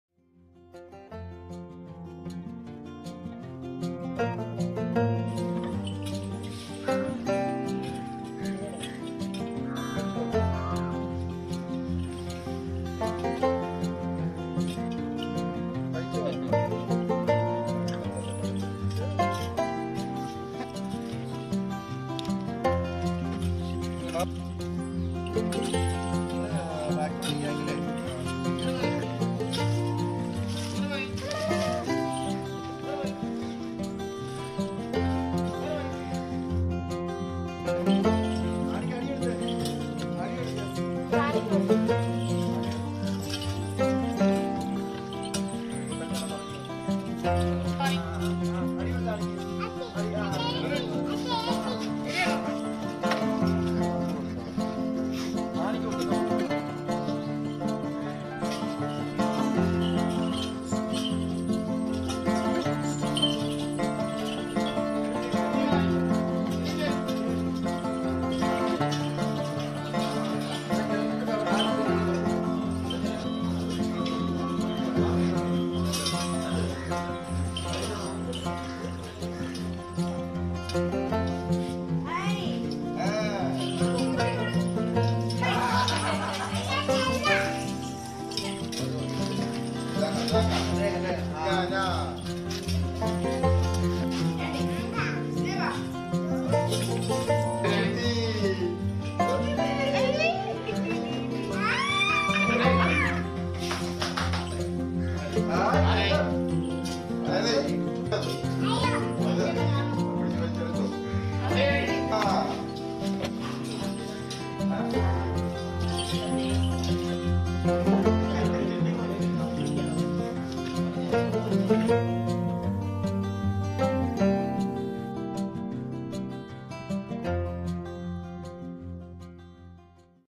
Tik Tok video on friendship between elephant, child goes viral on social media
A tik tok video on the friendship between a child and elephant went viral on social media from Thiruvananthapuram. The elephant is recordically above 31 years of age and is a member of the family.